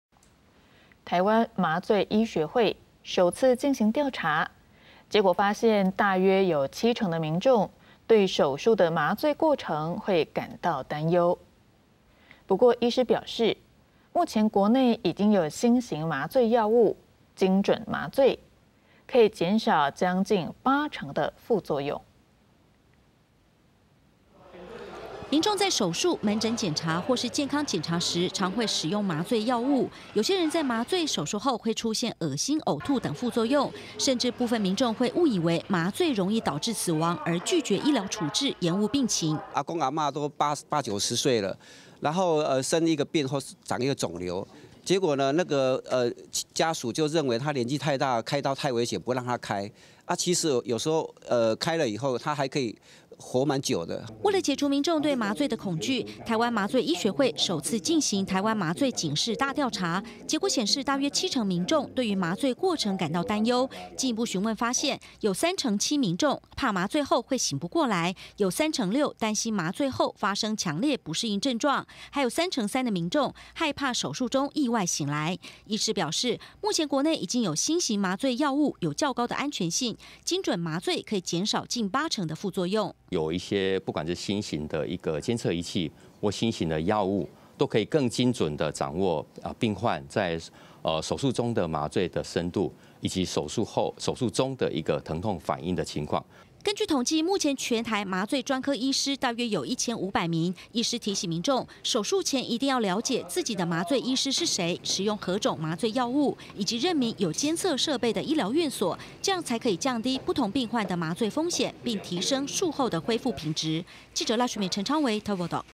調查指出 7成民眾對手術麻醉過程擔憂 | 20231122公視手語新聞
台灣"麻醉醫學會"，首次進行調查，結果發現、大約有七成民眾、對手術的"麻醉過程"感到擔憂。不過醫師表示，目前國內，已經有"新型麻醉"藥物，精準麻醉，可以減少"近八成的"副作用。
🙌每週一到週五 早上8:00-8:30 歡迎收看